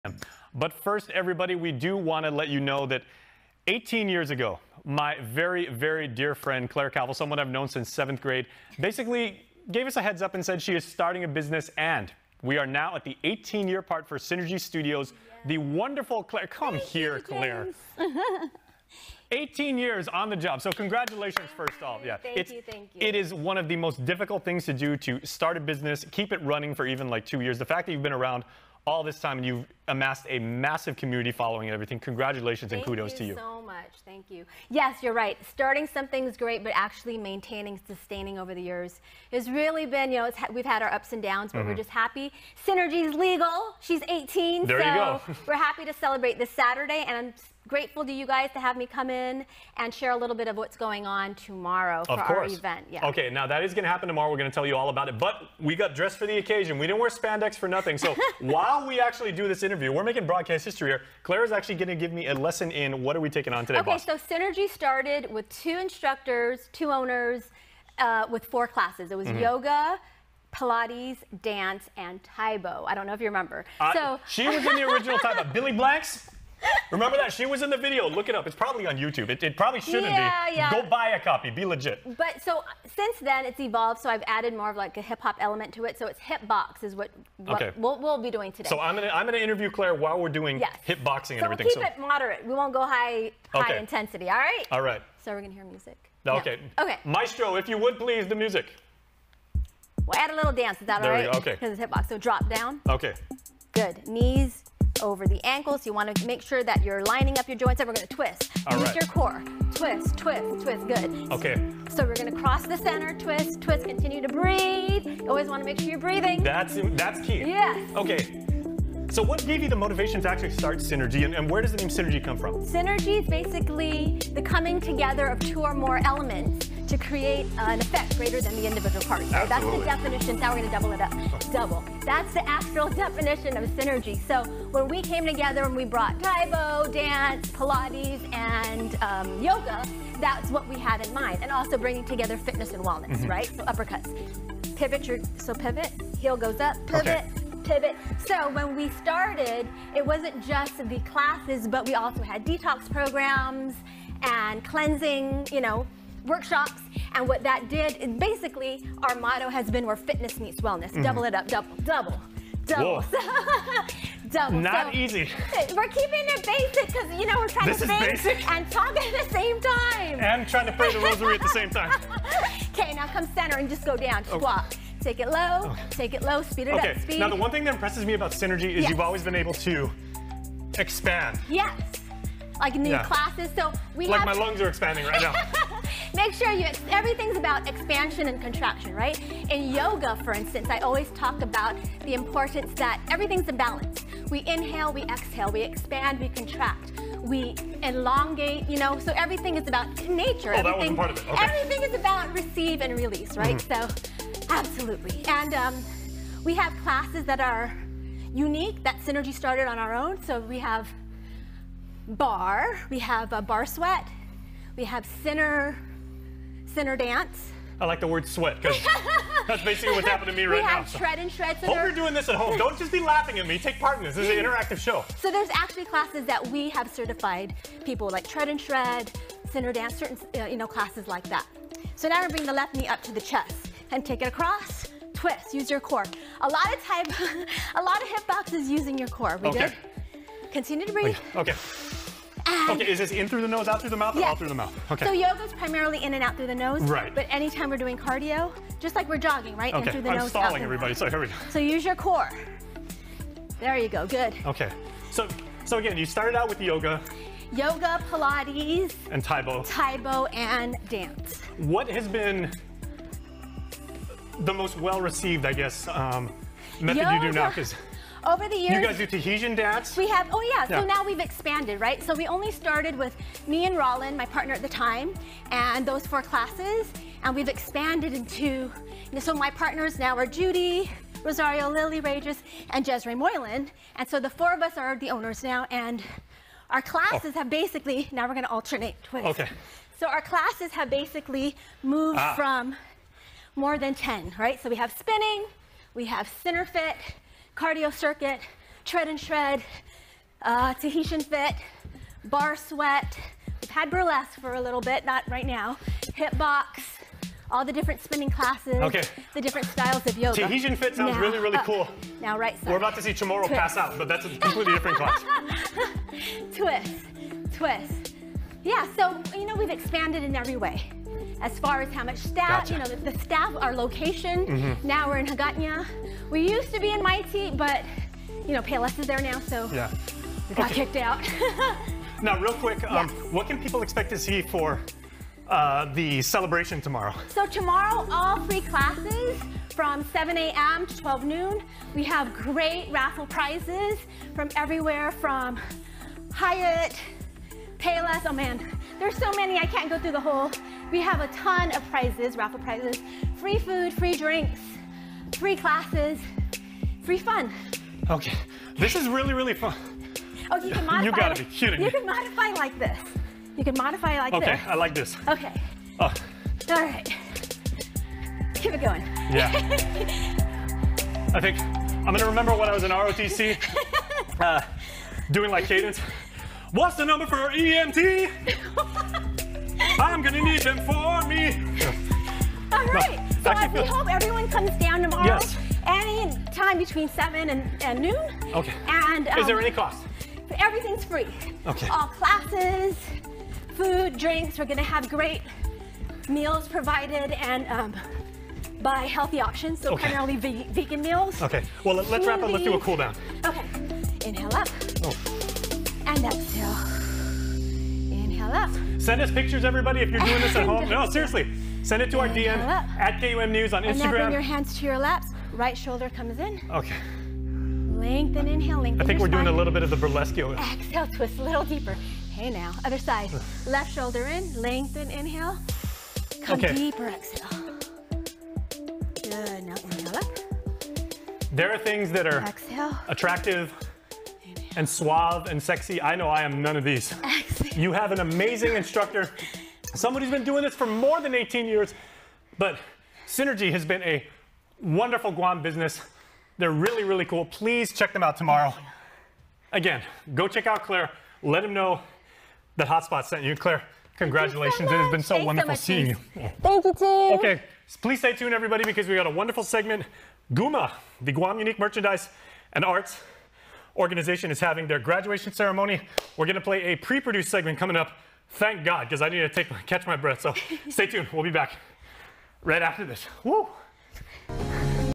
Clare gives Jason a hip-box lesson - MID-INTERVIEW! - to celebrate Synergy Studio's 18th anniversary
Clare Calvo gives Jason a hip-box lesson - MID-INTERVIEW! - to celebrate Synergy Studio's 18th anniversary